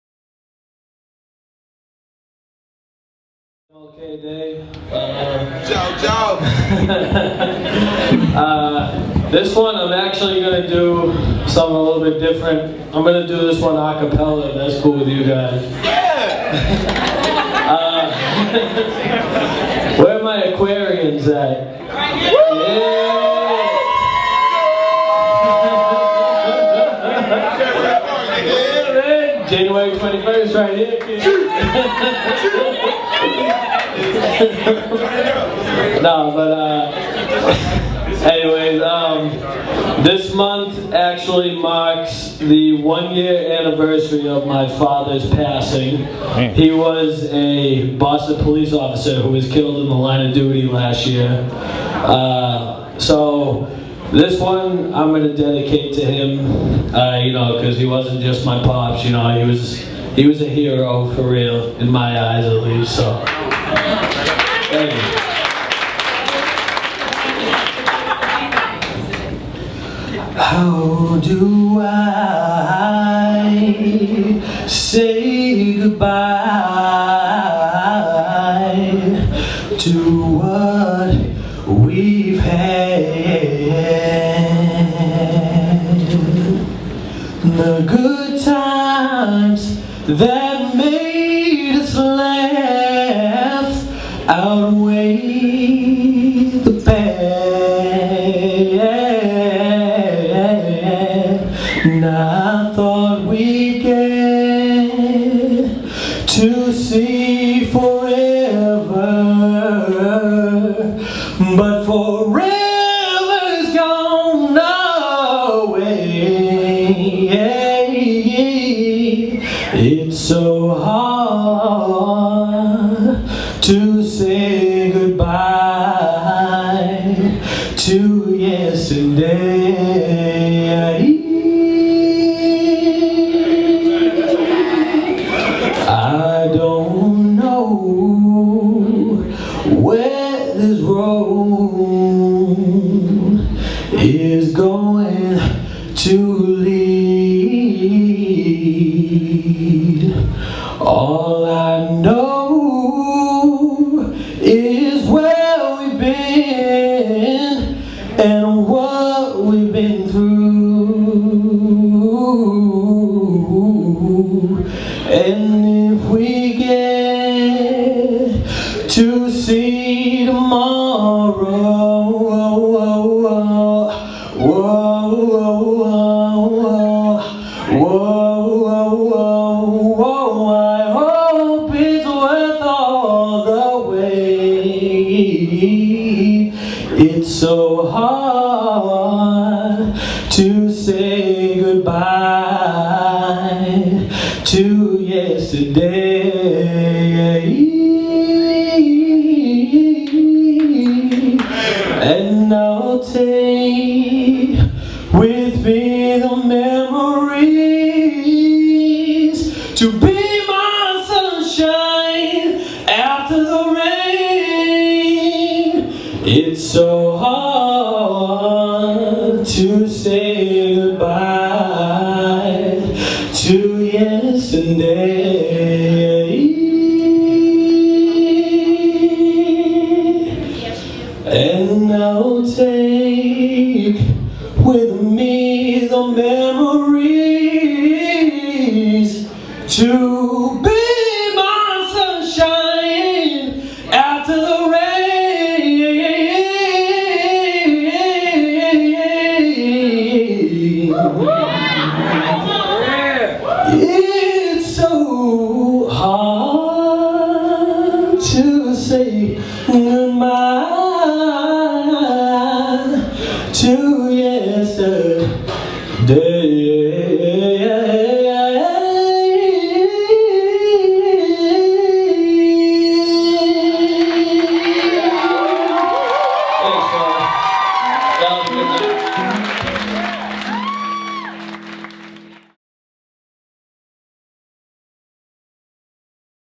Soul Sessions KC - Joe Gallant
Joe dedicates a performance to his late father

Shot with:
Canon t3i, 18-55mm lens, Rode Video Mic